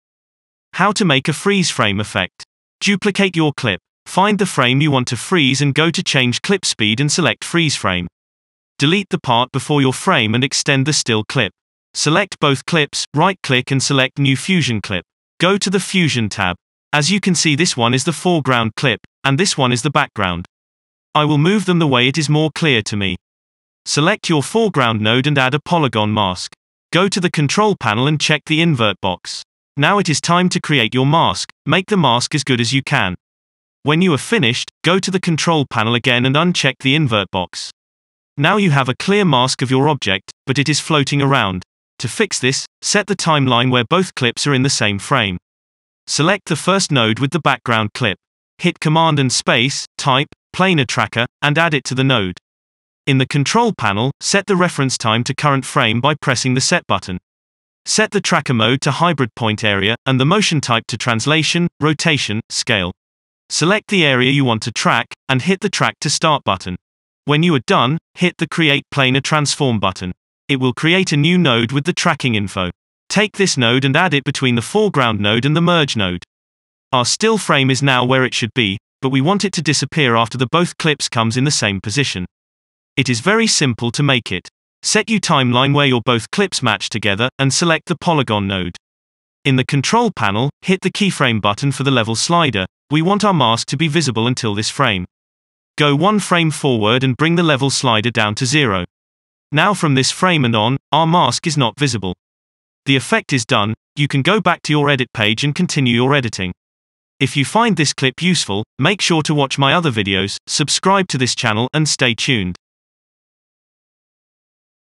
How To Make The Freeze Frame Effect | DaVinci Resolve 17 | Lazy Way
How to make the freeze frame effect, that is used in the jack black viral video, inside davinci resolve, in a few simple steps.

If you are thinking to buy a new camera you can try one of those great cameras :
Sony Alpha 7S III
Blackmagic pocket 6K
sony a6500